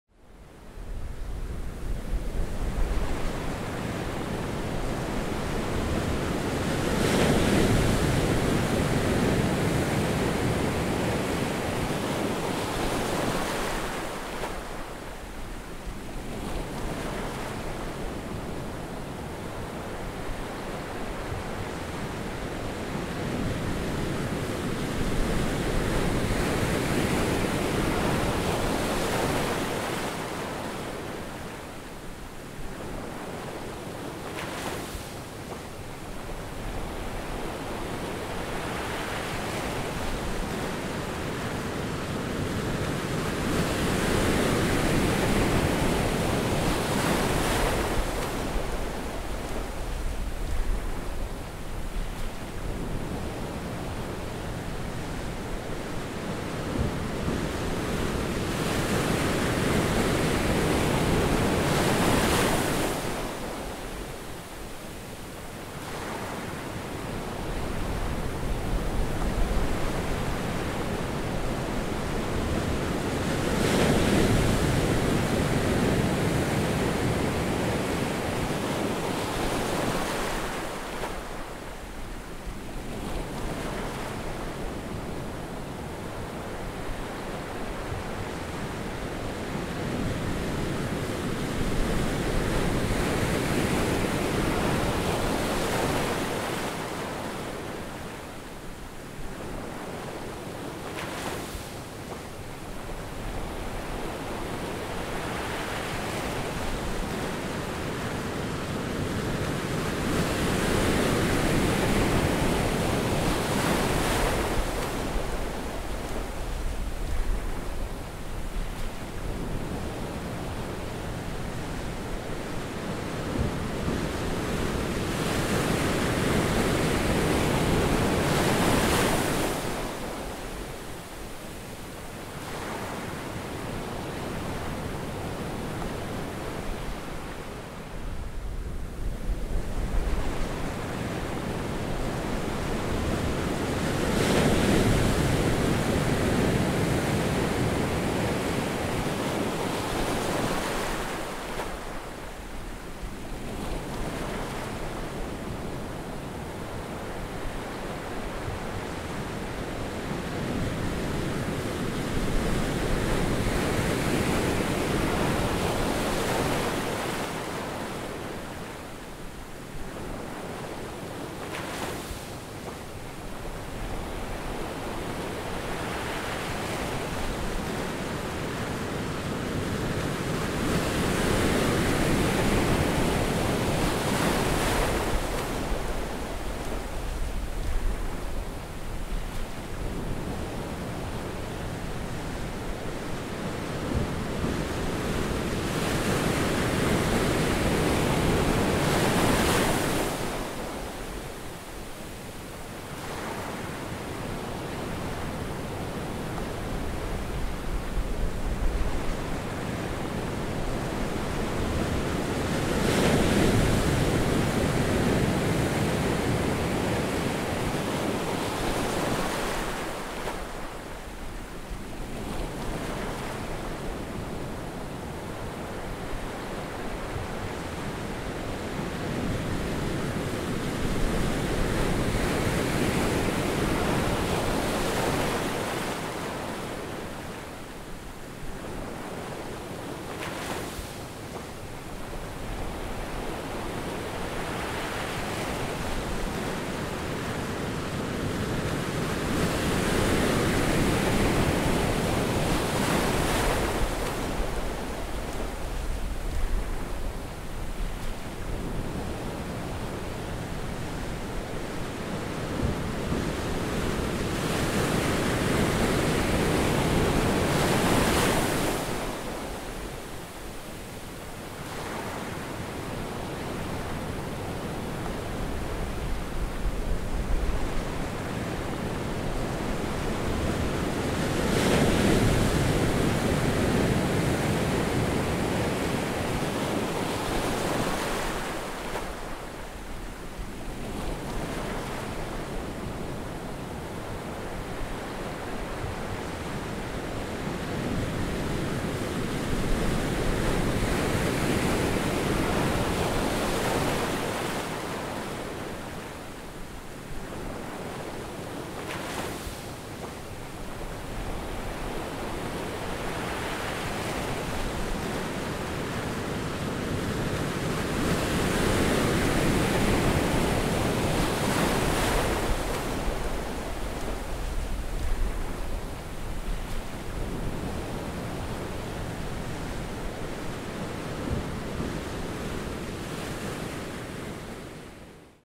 A Moonlit Spectacle | Green Turtles Nest On Ascension Island
Filmed using a highly sensitive low-light camera, watch the moonlit nesting process of a Green turtle on Long Beach, Ascension Island. Peer through the darkness and learn about the life history of these incredible animals in a process which hasn't changed for millions of years. Made for the Ascension Island Government Conservation and Fisheries Directorate.